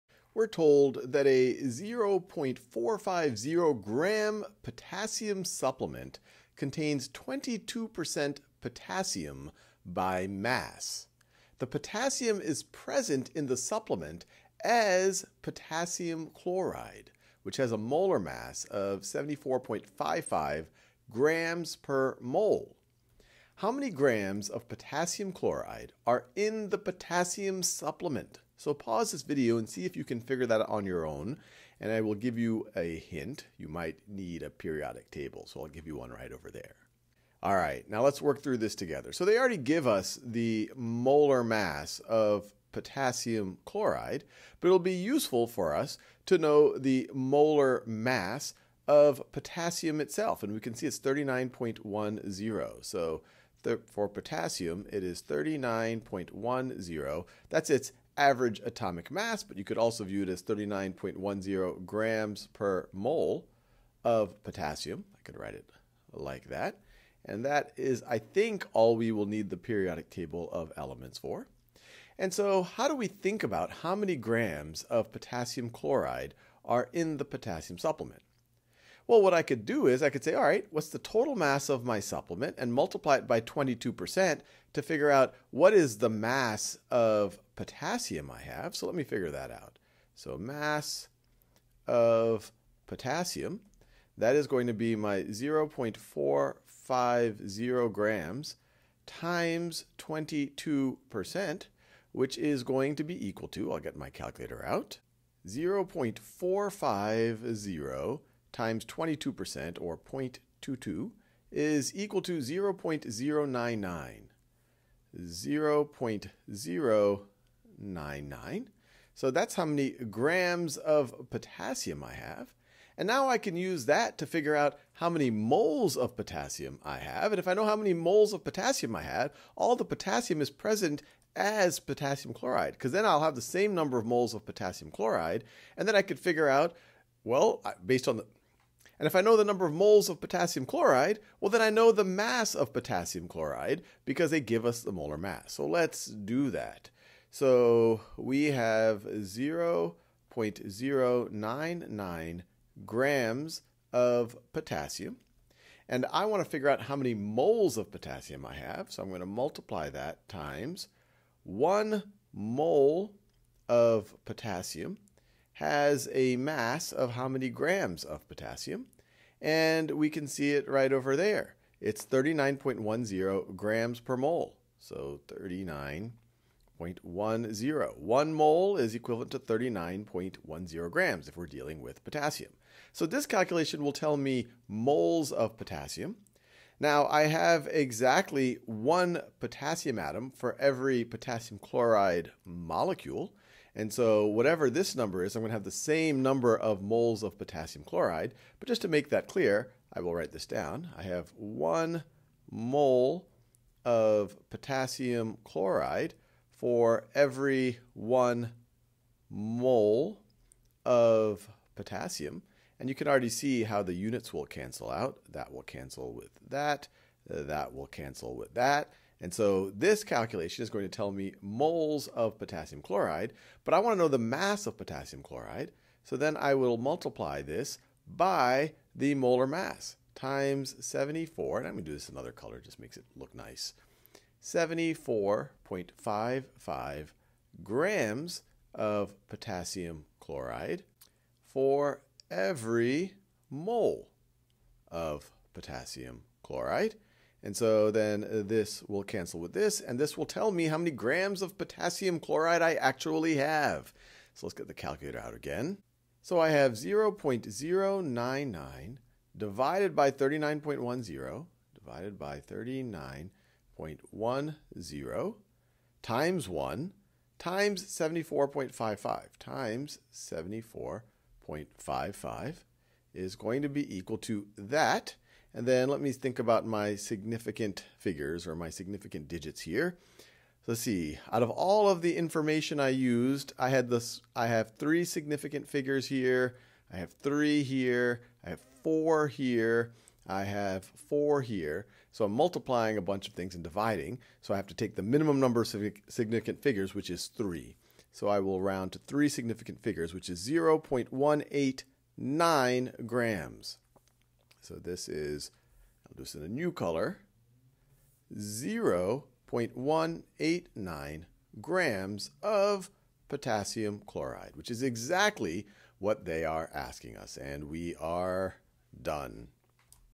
Worked example: Calculating the mass of a substance in a mixture | AP Chemistry | Khan Academy
Elemental analysis can be used to determine the amounts of substances in a mixture. For example, if elemental analysis tells us that a potassium supplement contains 22% K by mass, and we know that the K is present as KCl, we can calculate the grams of KCl in the supplement. View more lessons or practice this subject